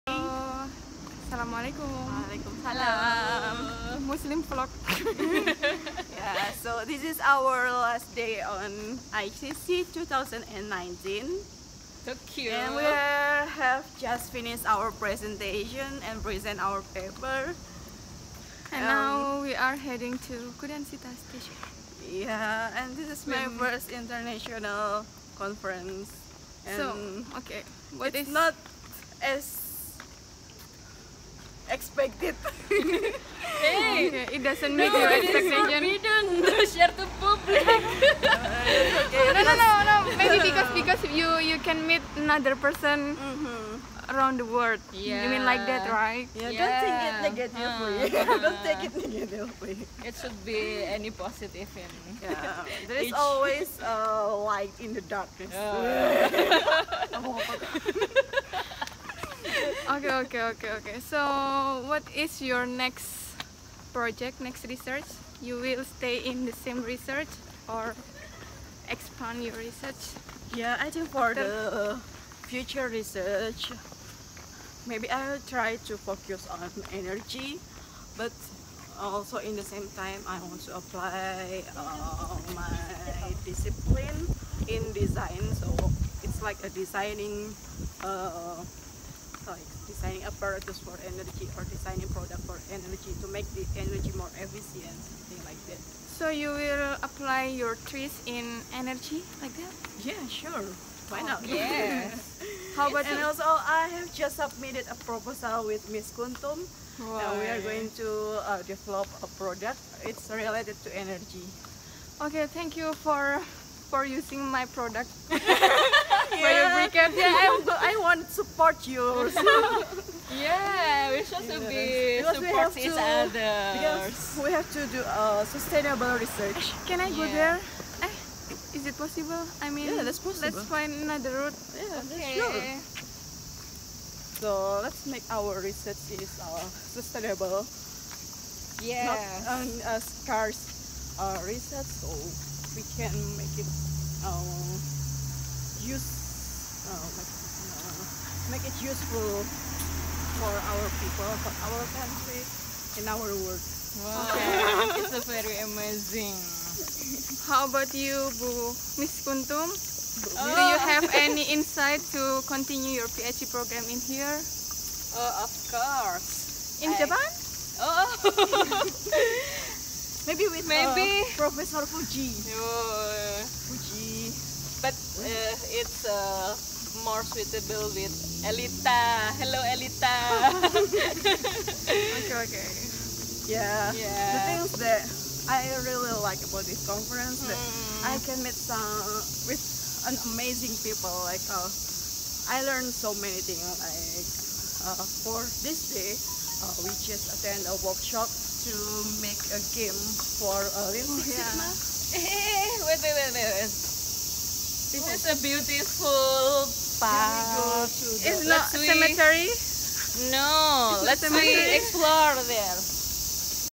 Joining International Conference, Yay or Nay?
We were lectures in Universitas Internasional Semen Indonesia attending an international conference in Tokyo University of Science. Each of us were sharing about how did we get after coming to the conference.
Check this video if you want to know the detail. 😄